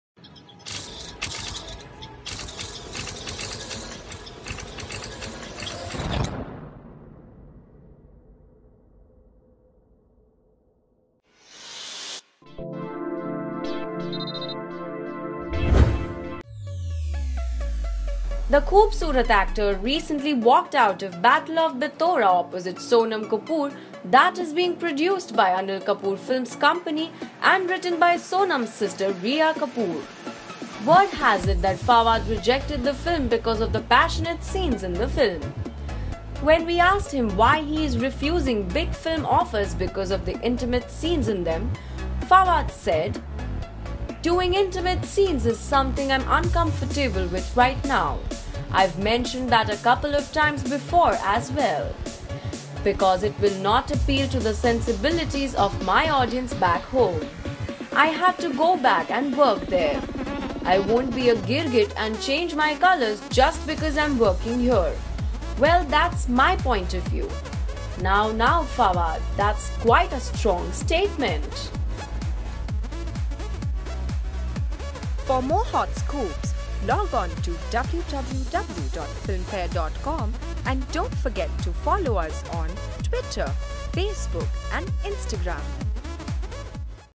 The real reason behind Fawad Khan's exit from Battle For Bittora
The dishy actor from Pakistan apparently walked out of Battle For Bittora opposite Sonam Kapoor because of the steamy scenes in it. Watch this video to know more.